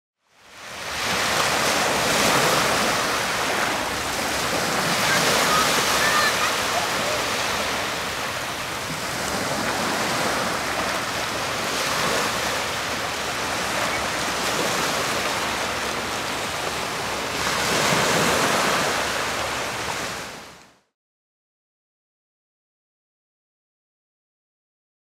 Aguas Cristalinas - Playa de La Mata (04/12/2018)
Aguas cristalinas en la Playa de La Mata en un día radiante con una temperatura máxima de 23ºC. Imágenes grabadas por Joaquín Carrión.